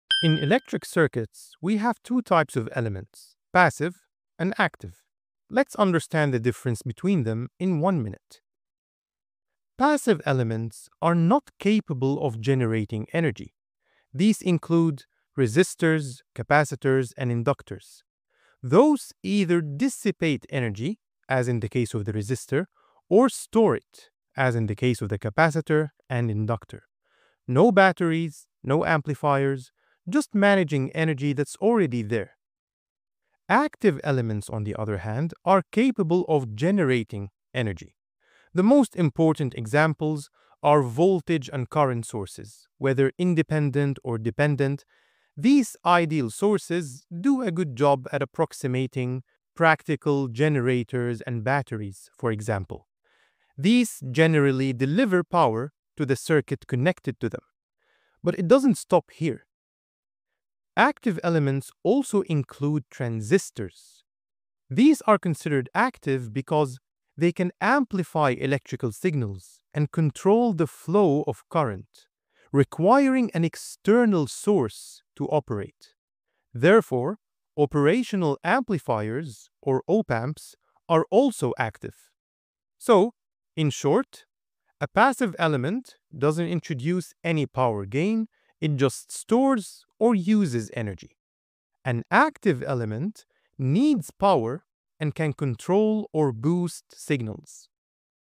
Passive vs Active Elements in Electric Circuits
In electric circuits, there are two types of elements: passive and active. Let’s understand the difference between them in 1 minute.